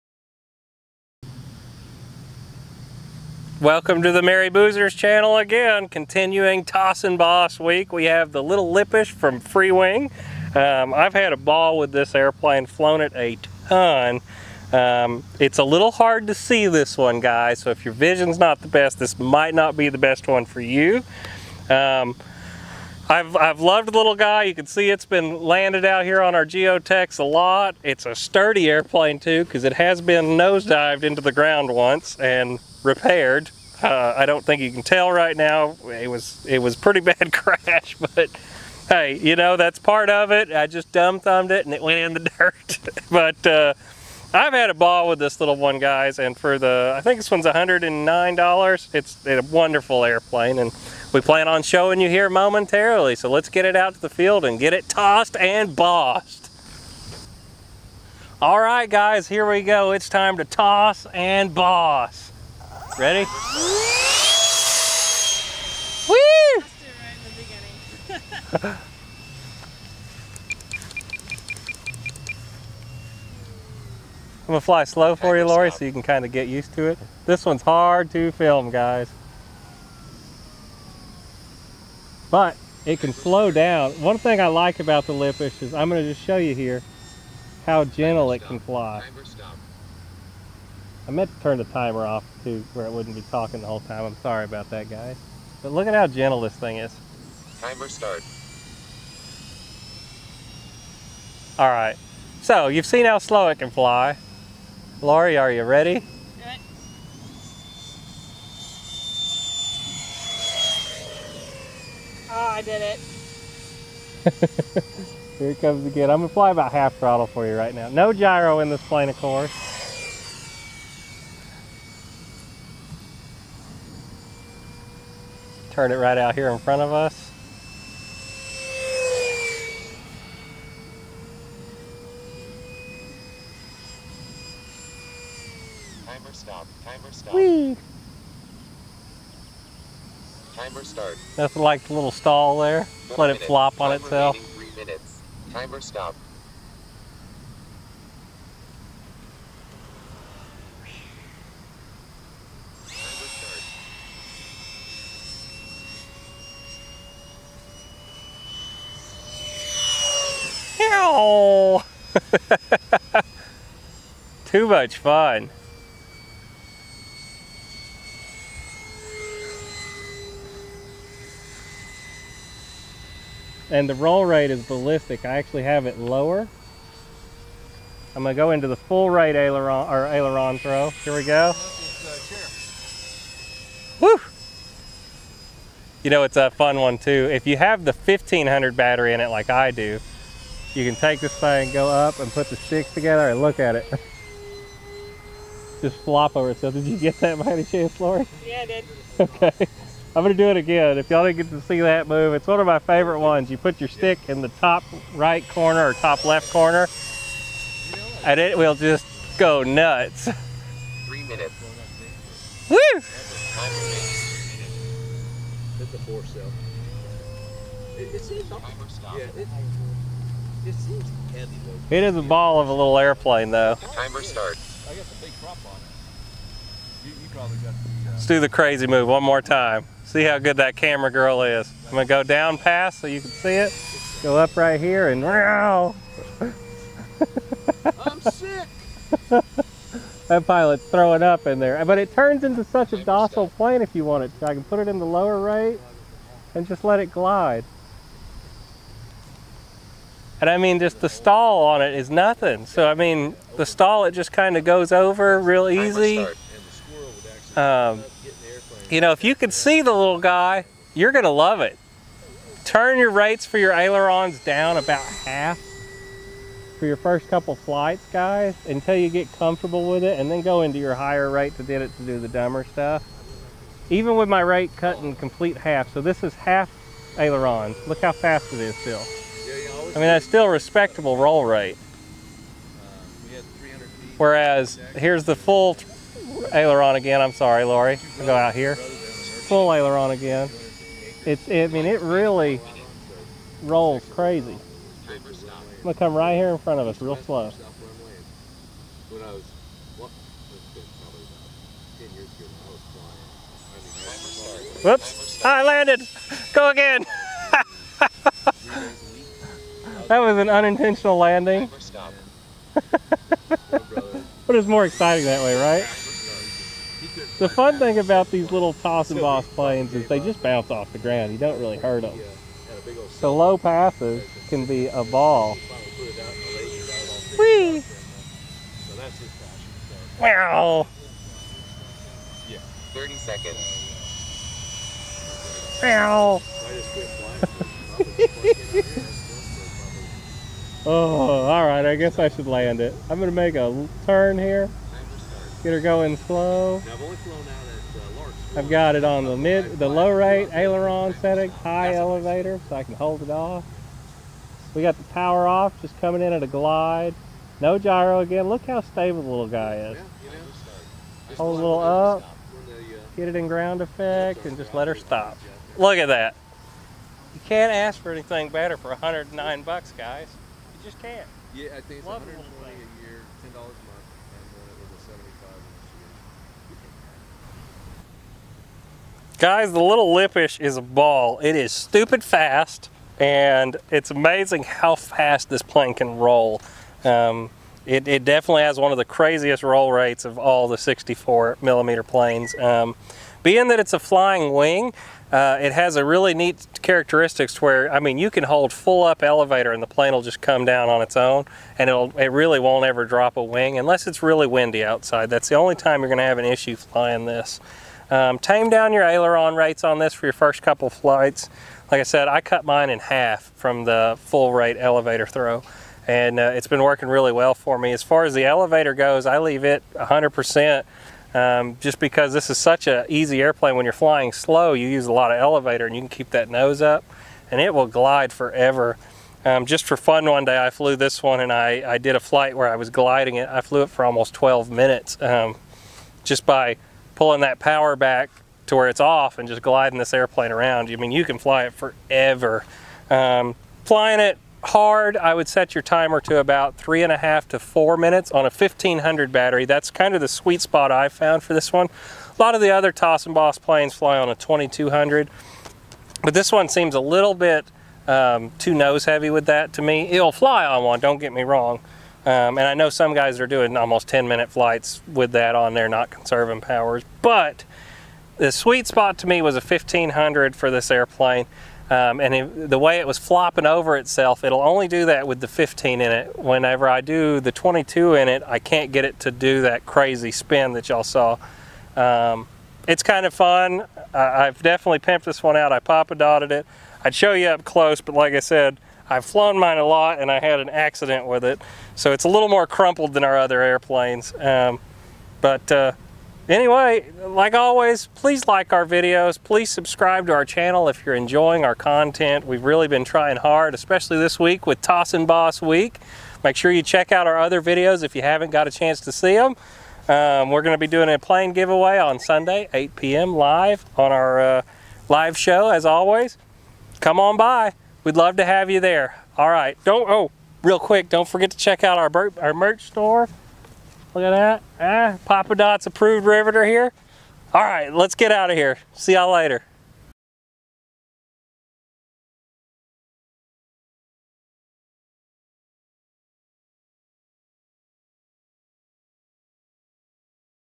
Freewing Lippisch 64mm Toss And Boss Week Special! This Thing Is Crazy Fast And Fun
Freewing Lippisch has a special place in my hanger. Actually boozer is probably buying a new one, I might have crashed it lol. Sometimes when your having to much fun the ground jumps out at your planes. anyway this plane can fly fast or slow. The stall speed is super slow and the stall is very gentle. Get one, you definitely will not regret it. We love seeing you here